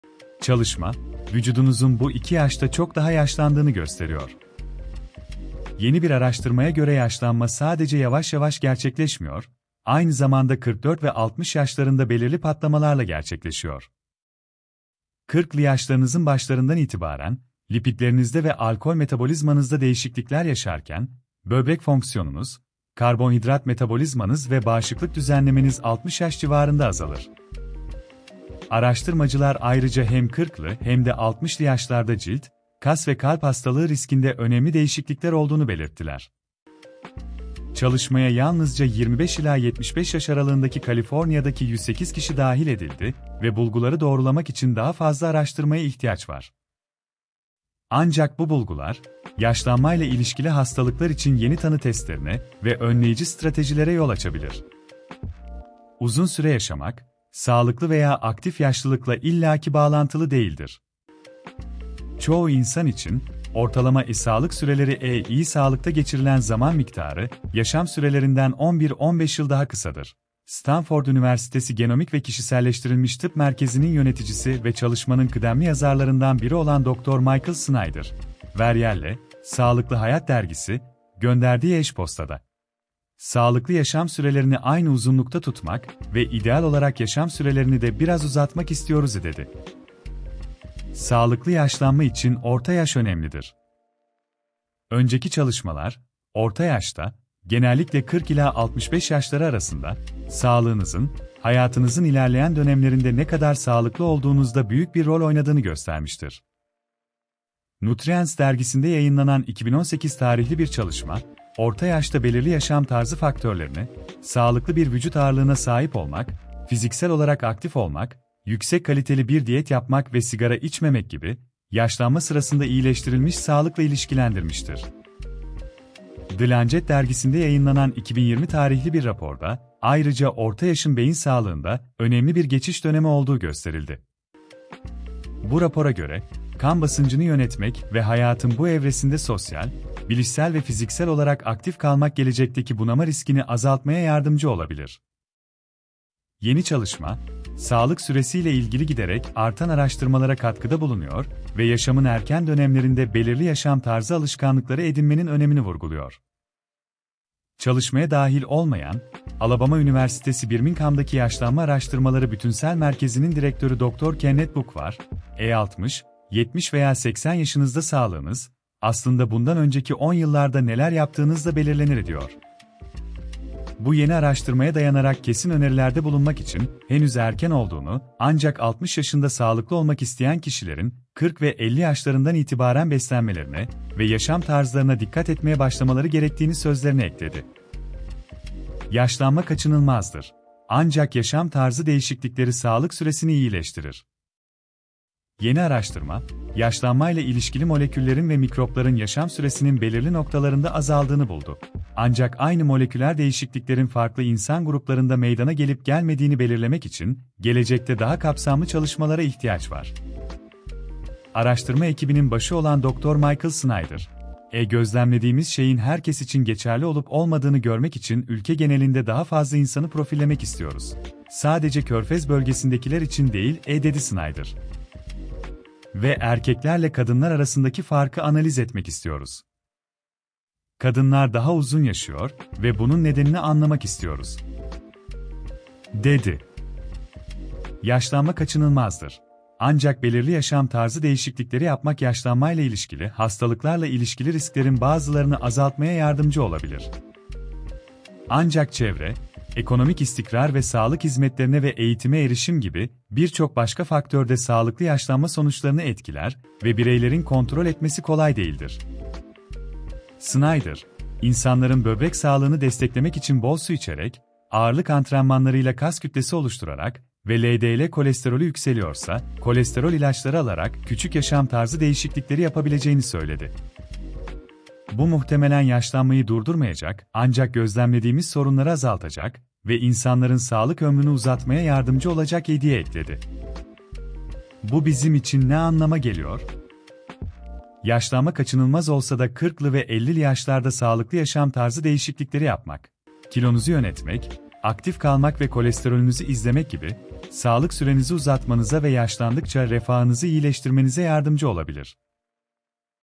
Çalışma, Vücudunuzun Bu İki Yaşta Çok Daha Yaşlandığını Gösteriyor
Yeni bir araştırmaya göre yaşlanma sadece yavaş yavaş gerçekleşmiyor, aynı zamanda 44 ve 60 yaşlarında belirli patlamalarla gerçekleşiyor.
40'lı yaşlarınızın başlarından itibaren, lipidlerinizde ve alkol metabolizmanızda değişiklikler yaşarken, böbrek fonksiyonunuz, karbonhidrat metabolizmanız ve bağışıklık düzenlemeniz 60 yaş civarında azalır. Araştırmacılar ayrıca hem 40'lı hem de 60'lı yaşlarda cilt, kas ve kalp hastalığı riskinde önemli değişiklikler olduğunu belirttiler.